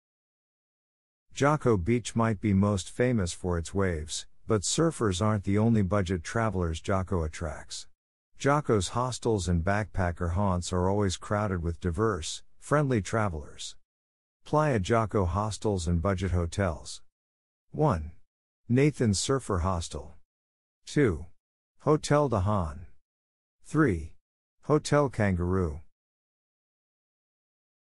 Playa Jaco
Jaco Beach might be most famous for its waves, but surfers aren't the only budget travelers Jaco attracts. Jaco's hostels and backpacker haunts are always crowded with diverse, friendly travelers..
Playa Jaco hostels and budget hotels:.
1. Nathon's Surfer Hostel.
2. Hotel De Haan.
3. Hotel Kangaroo.